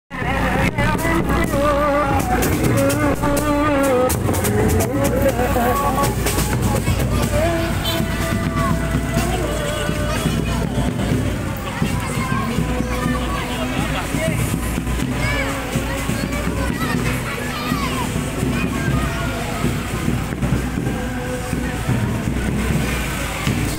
Pawai Obor dan Lampu Hias di Malam Takbiran Kecamatan Makassar kelurahan larangbangi
Video dari Garuda